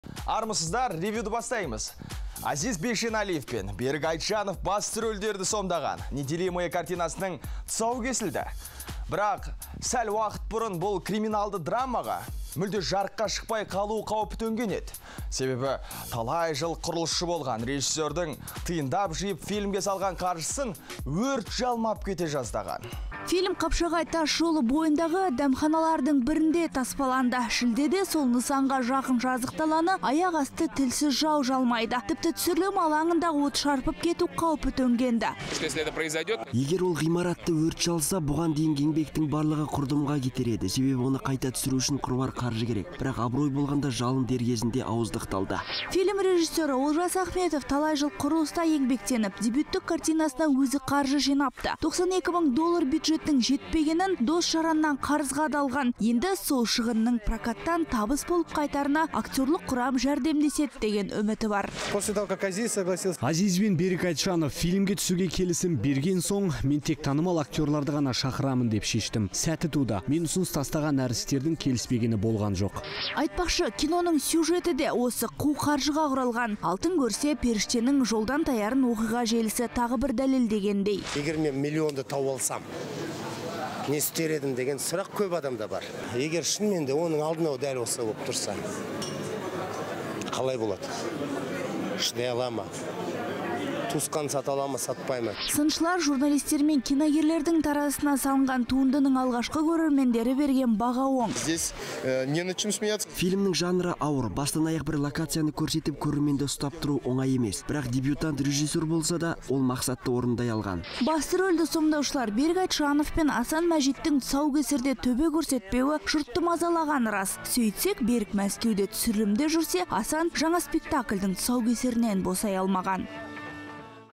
"Неделимое" картинасының тұсаукесері!
Азиз Бейшеналиев пен Берік Айтжанов басты рөлдерді сомдаған «Неделимое» картинасының тұсауы кесілді. Бірақ, сәл уақыт бұрын бұл криминалды драмаға, мүлде жарыққа шықпай қалу қаупі төнген еді. Себебі, талай жыл құрылысшы болған режиссердің, тиындап жиып фильмге салған қаржысын өрт жалмап кете жаздаған.